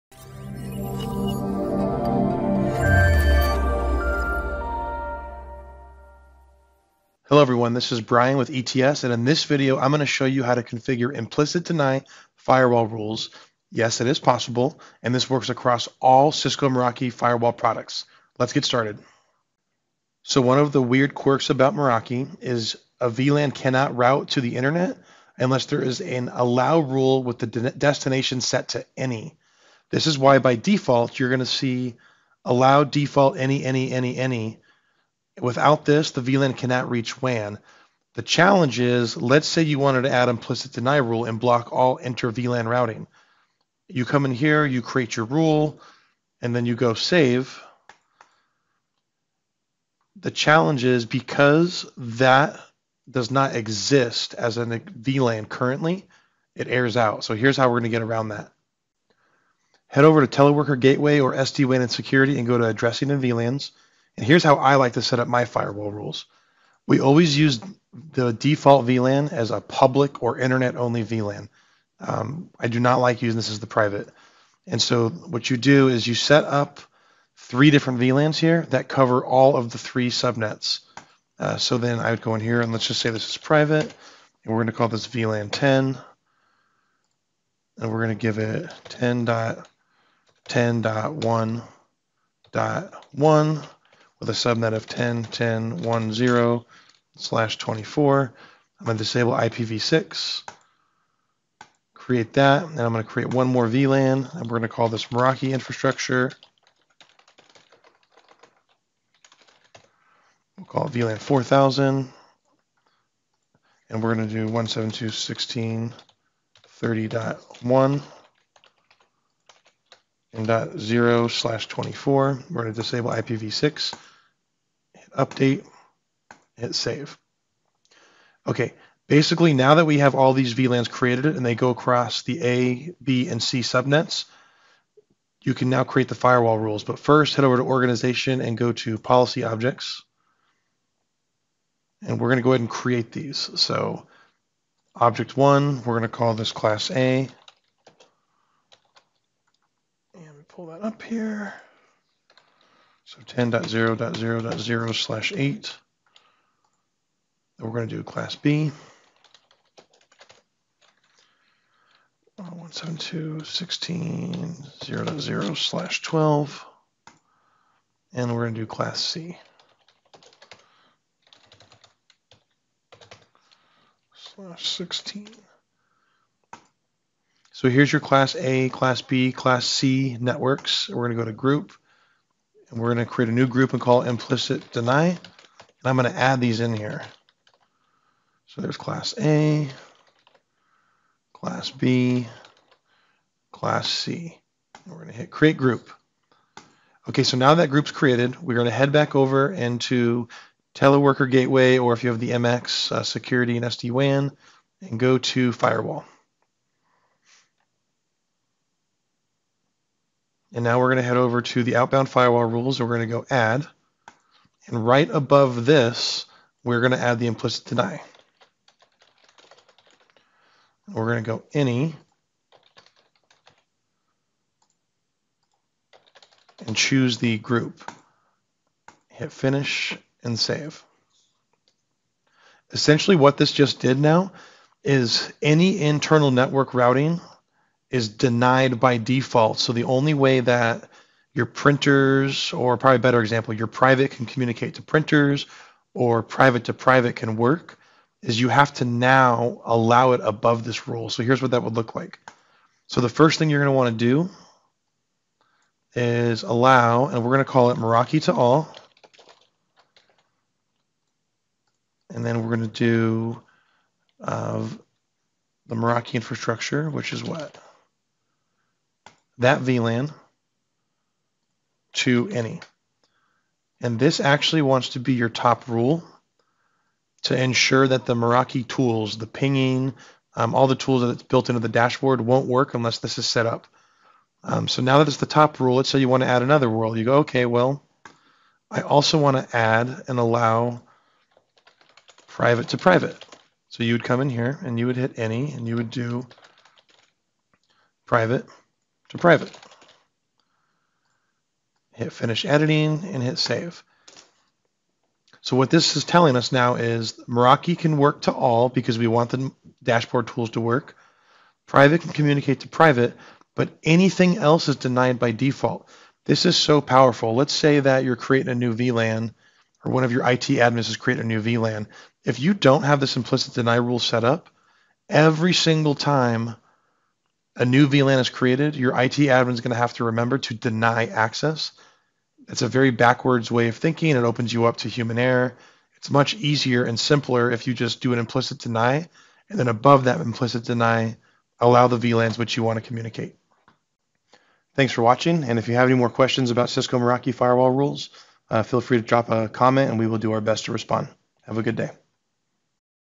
Implicit Deny firewall rules with Cisco Meraki MX/Z3
This video will show you how to setup Cisco Meraki firewall rules with implicit deny that automatically blocks all inter-VLAN routing.